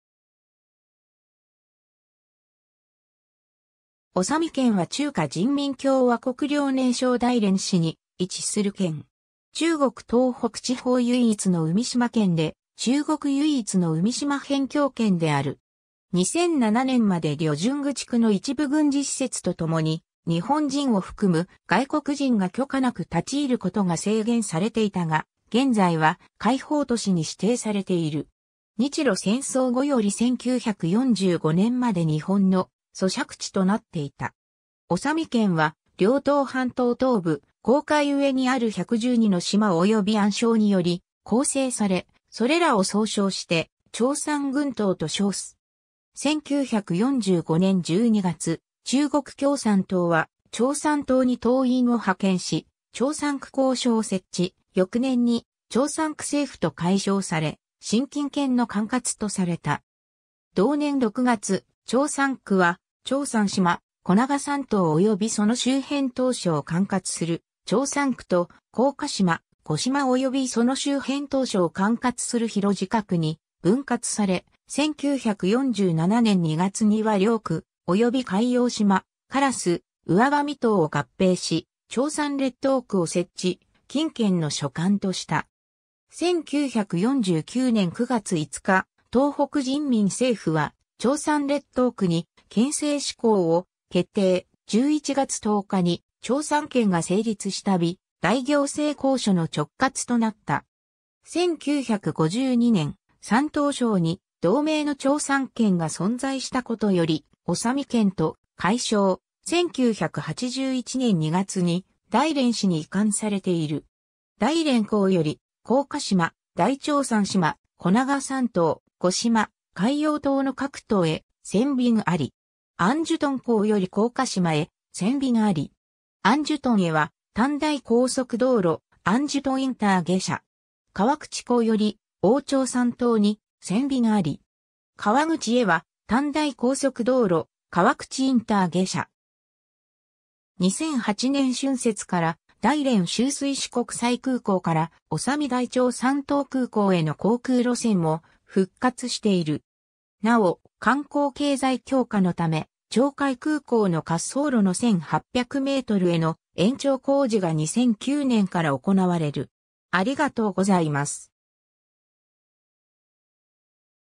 長海県
長海県（ちょうかい-けん）は中華人民共和国遼寧省大連市に位置する県。
中国東北地方唯一の海島県で、中国唯一の海島辺境県である。
 2007年まで旅順口区の一部軍事施設とともに、日本人を含む外国人が許可無く立ち入ることが制限されていたが、現在は開放都市に指定されている。
日露戦争後より1945年まで日本の租借地となっていた。
 長海県は遼東半島東部・黄海上にある112の島及び暗礁により構成され、それらを総称して長山群島と称す。
 1945年12月、中国共産党は長山島に党員を派遣し長山区公所を設置、翌年に長山区政府と改称され新金県の管轄とされた。
同年6月、長山区は長山島、小長山島及びその周辺島嶼を管轄する長山区と、広鹿島、獐子島及びその周辺島嶼を管轄する広鹿区に分割され、1947年2月には両区及び海洋島、烏蟒島を合併し長山列島区を設置、金県の所管とした。
 1949年9月5日、東北人民政府は長山列島区に県制施行を決定、11月10日に長山県が成立し旅大行政公署の直轄となった。
1952年、山東省に同名の長山県が存在したことより長海県と改称、1981年2月に大連市に移管されている。
 大連港より、広鹿島、大長山島、小長山島、獐子島、海洋島の各島へ船便あり。
 杏樹屯港（大連市金州区）より広鹿島へ船便あり。
杏樹屯へは、丹大高速道路・杏樹屯インター下車。
 皮口港（大連市普蘭店区）より大長山島に船便あり。
皮口へは、丹大高速道路・皮口インター下車。
 2008年春節から大連周水子国際空港から長海大長山島空港への航空路線も復活している。
なお、観光経済強化のため、長海空港の滑走路の1800メートルへの延長工事が2009年から行われる。